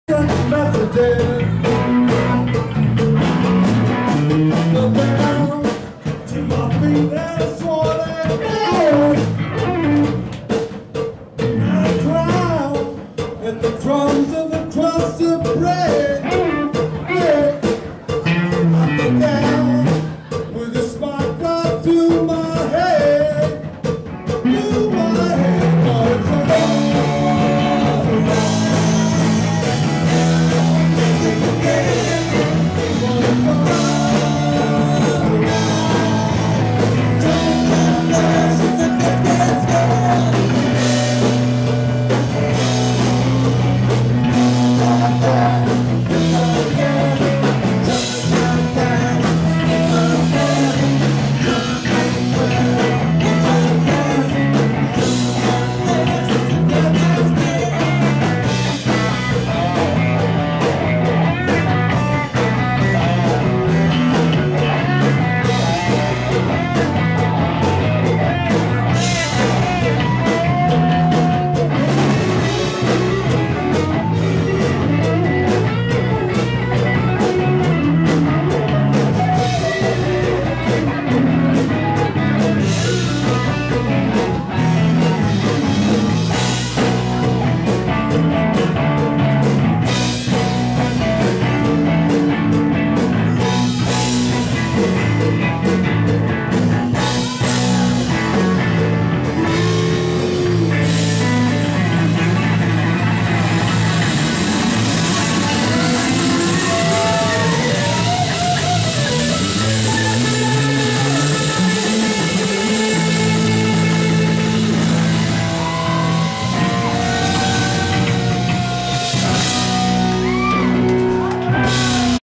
Dancing Jammies
1st Annual Pajama Party at Mineral Springs Bar & Grill, Easton PA...shake those jammies ;)